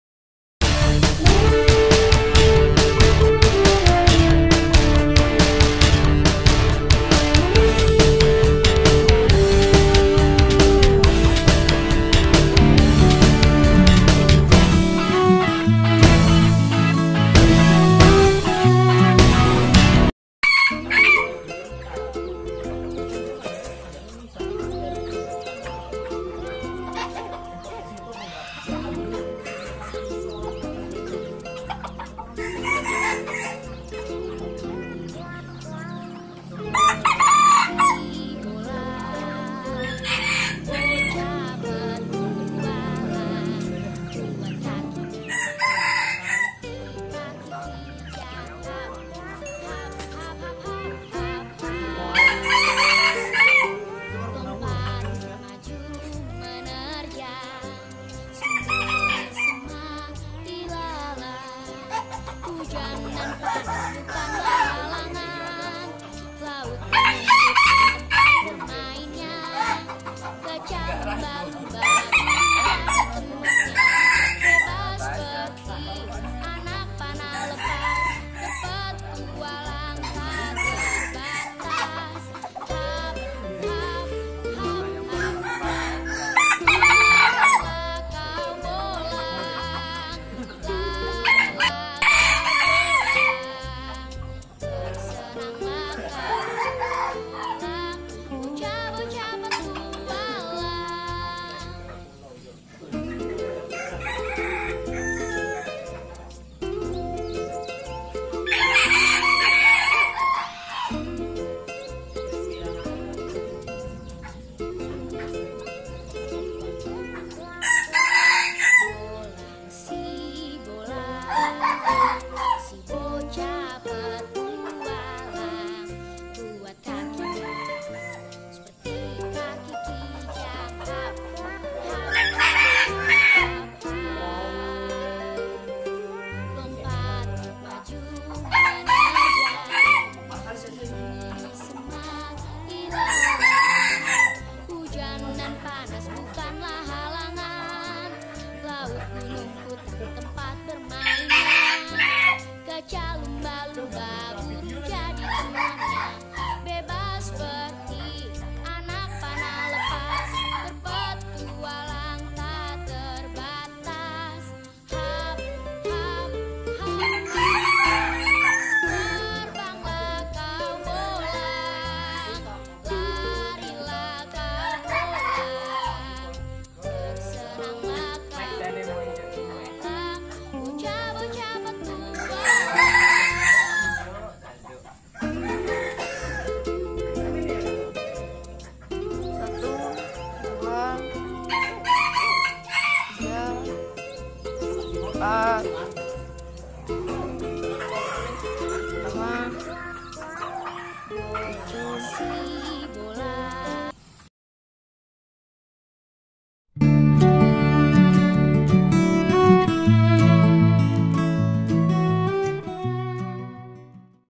SEPARING CALON DAN PIKAT AYAM HUTAN / bersama anggota grup seni mikat trans Kuansing
separing kokokayam,red junglefowl, chicken, chicken flying